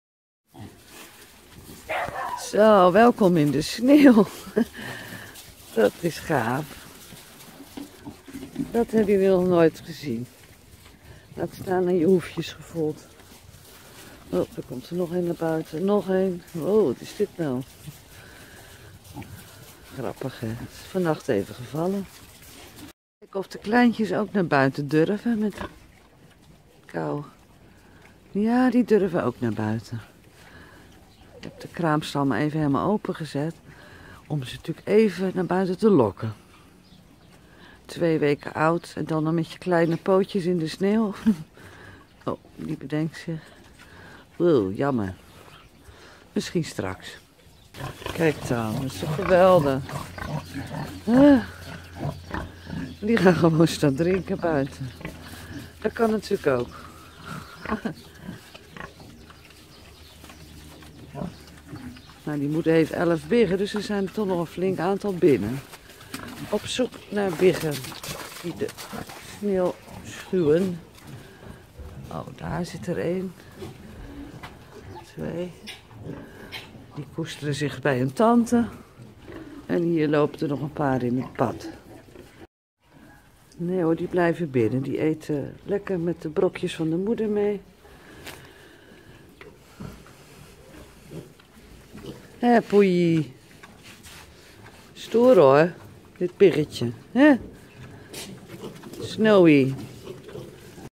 Vijfsterrenvlog 15 - Biggen in de sneeuw
Er is sneeuw gevallen op de boerderij van Diana! En dat is nieuw voor de jonge varkens. De oudere biggen durven wel naar buiten, maar de biggetjes van twee weken oud vinden het behóórlijk spannend.